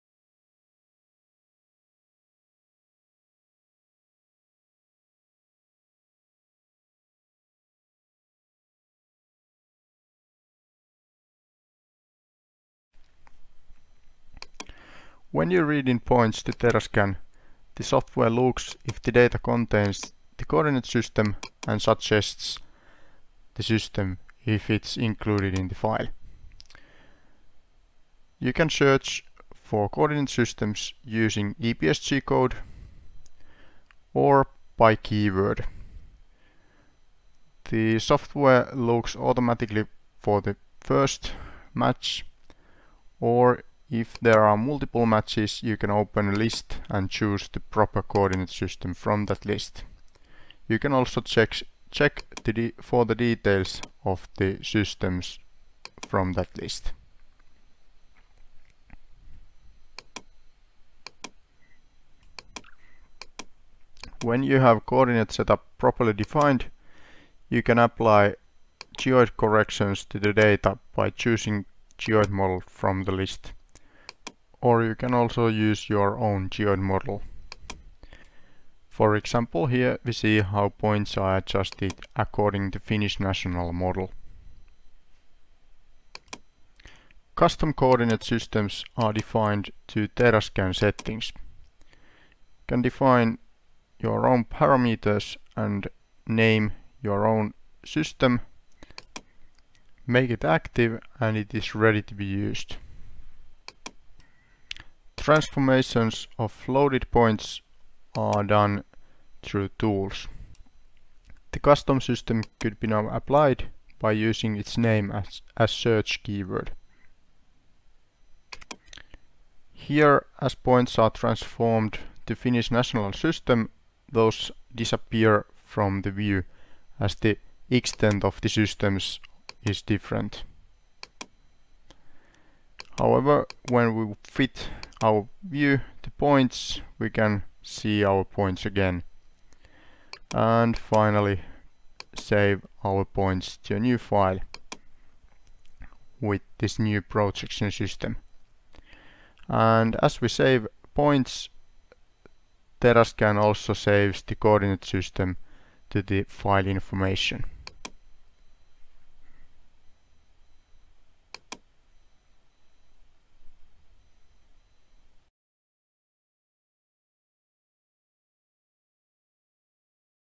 Introduction to Coordinate Systems and Projections in Terrasolid Applications
A quick introduction to the management of projection systems in Terrasolid applications, demonstrated with TerraScan.
The software has most of the available systems built-in, applicable by searching with EPSG code.

Video chapters:
0:00 Intro
0:12 Transform data during read-in
1:02 Adjust elevations to geoid
1:25 Define custom projection systems
1:43 Transform loaded points to another projection system
2:18 Save points to file stores active projection system